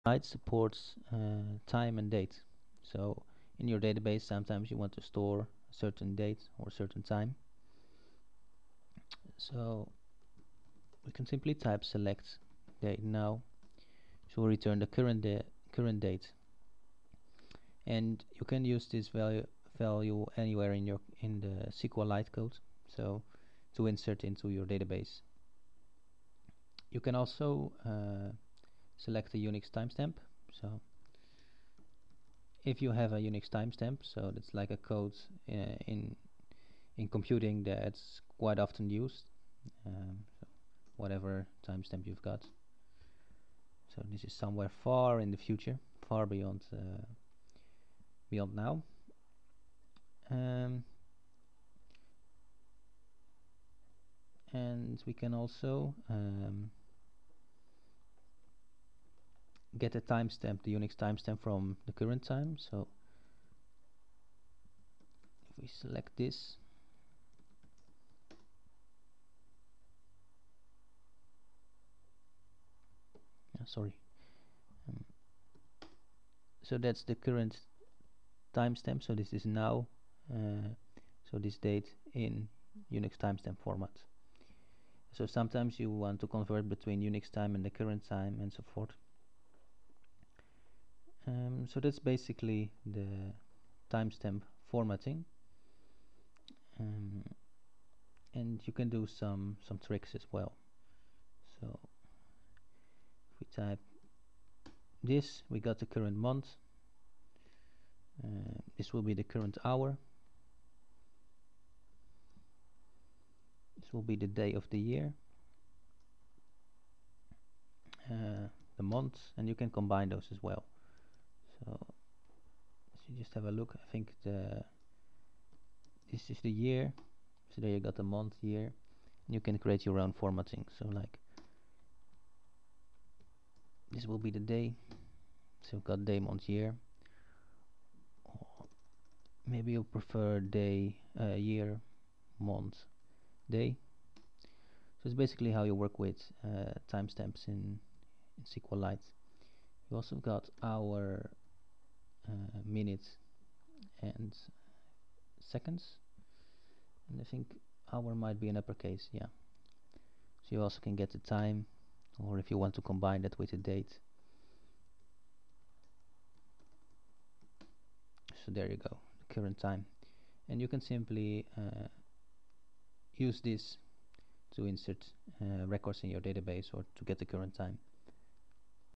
SQLite - Date and time
Learn how to use date and time in SQLite.